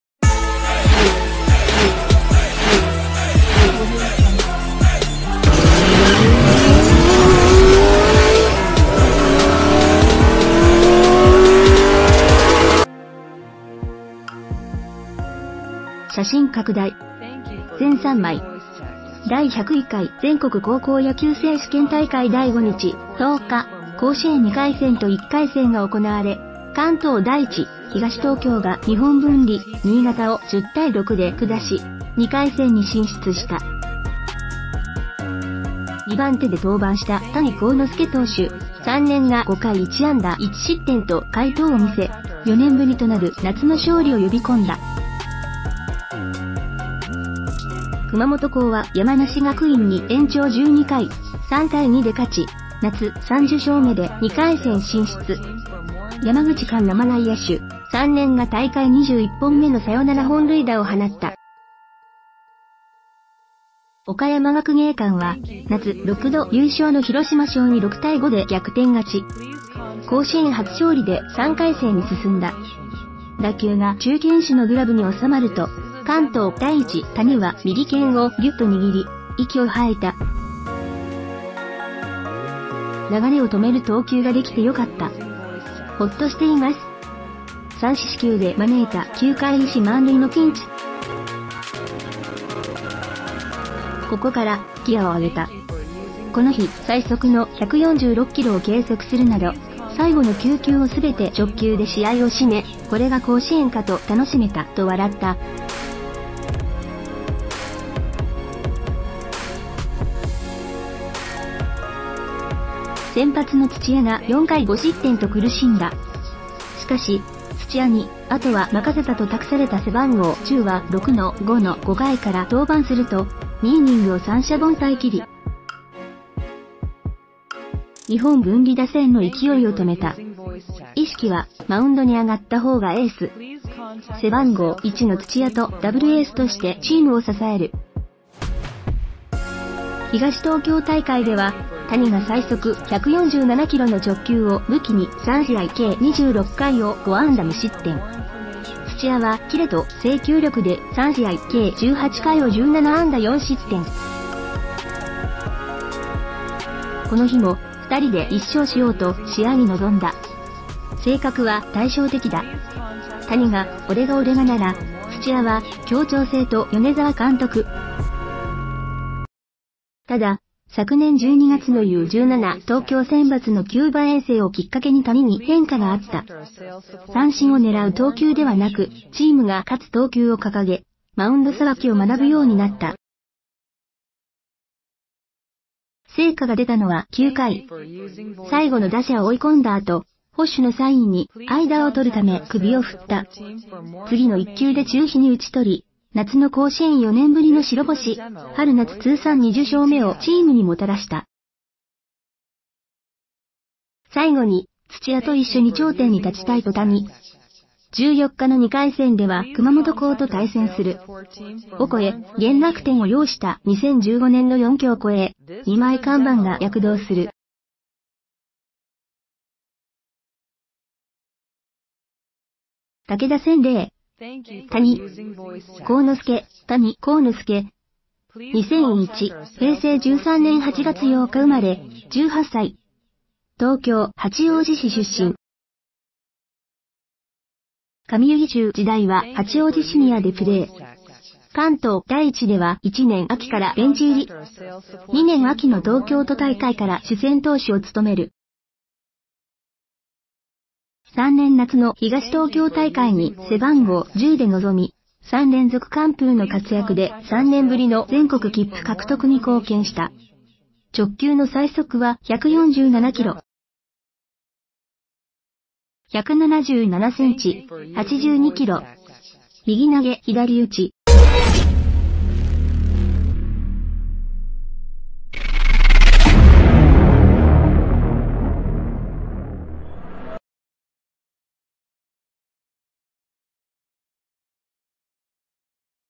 関東第一、春夏通算２０勝！谷が五回からロング救援１失点甲子園Top News
,News,関東第一、春夏通算２０勝！谷が五回からロング救援１失点甲子園Top
関東第一、春夏通算２０勝！谷が五回からロング救援１失点甲子園Top News ► Subscribe for more 写真拡大 (全3枚) 第１０１回全国高校野球選手権大会第５日（１０日、甲子園）２回戦と１回戦が行われ、関東第一（東東京）が日本文理（新潟）を１０－６で下し、２回戦に進出した。２番手で登板した谷幸之